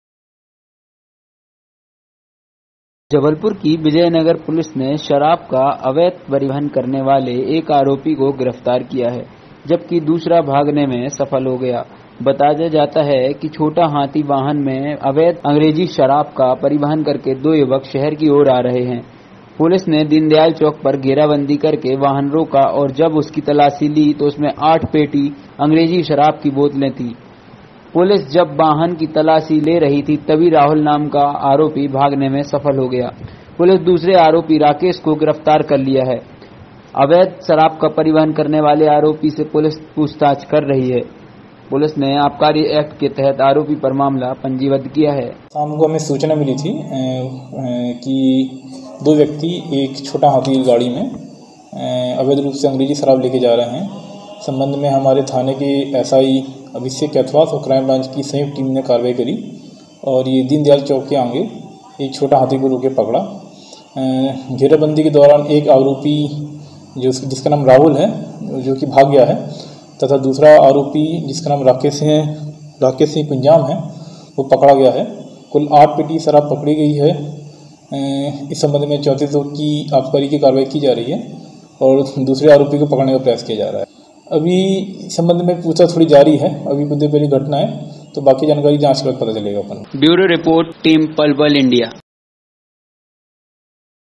दीनदयाल चौक के पास पकड़ी शराब | Pal Pal India News
जबलपूर की विजय नगर पुलिस ने  शराब का अवैध परिवहन करने वाले एक आरोपी को गिरफ्तार किया है जबकि दूसरा भागने में सफल हो गया। बताया जाता है कि छोटा हाथी वाहन में अवैध अंग्रेज़ी शराब का परिवहन करके दो युवक शहर की ओर आ रहे है। पुलिस ने दीनदयाल चौक पर घेरा बंदी करके वाहन रोका और जब उसकी तलाशी ली तो उसमें 8 पेटी अंग्रेजी शराब की बोतलें थी। पुलिस जब वाहन की तलाशी ली रही थी तभी राहुल नाम का आरोपी भागने में सफल हो गया। पुलिस दूसरे आरोपी राकेश को गिरफ्तार कर लिया है। अवैध शराब का परिवहन करने वाले आरोपी से पुलिस पूछताछ कर रही है। पुलिस ने आबकारी एक्ट के तहत आरोपी  पर मामला पंजीबद्ध किया है।